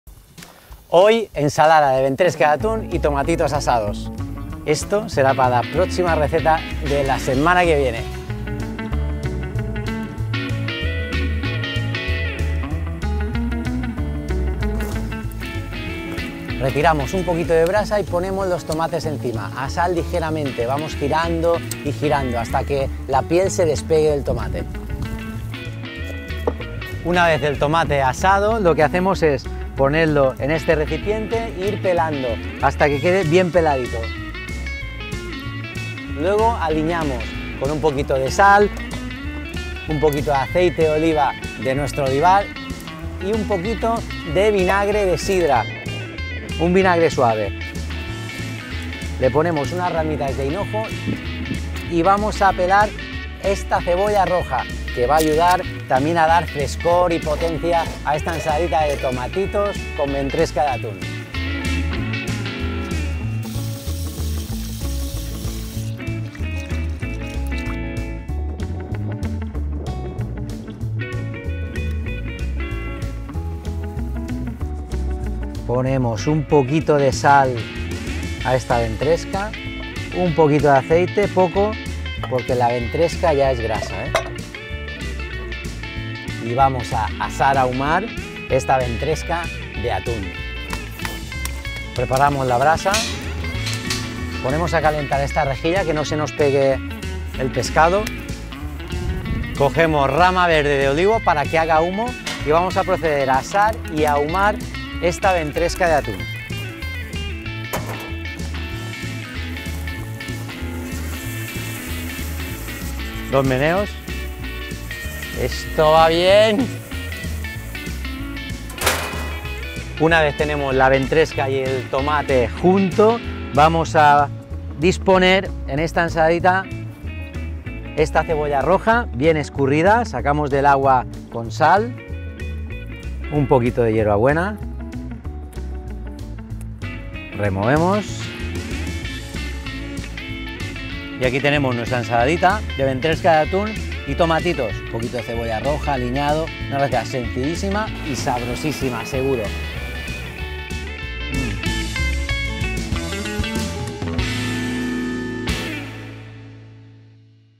COMO PREPARAR UNA ENSALADA DE VENTRESCA DE ATÚN
¡Se acerca el verano! Y con él traemos nuevas recetas, frescas y fáciles de preparar.

Esta vez Santi Taura nos sorprende con una ensalada de ventresca de atún y tomatitos asados. Si no tenéis plan para es fin de semana, ya sabéis con qué podéis sorprender a vuestros amigos o familiares.

Summer is coming! We bring new recipes, fresh and easy to prepare.

This time Santi Taura surprises us with a salad of tuna belly and roasted cherry tomatoes. If you do not have any plan for the weekend, you already know how you could surprise your friends or family.